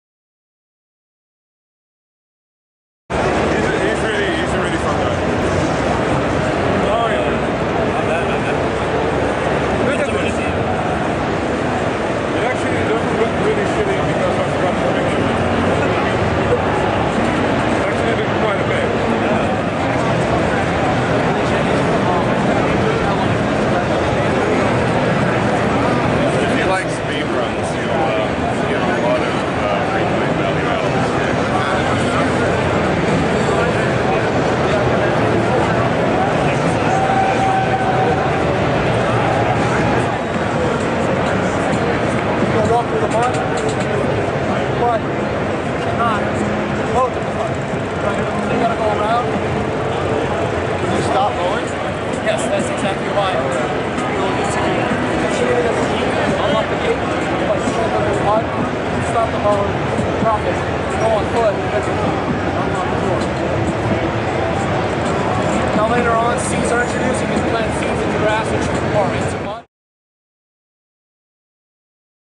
Indie Showcase - PAX East 2012
A quick look at three of the more interesting games found in the Indie Showcase.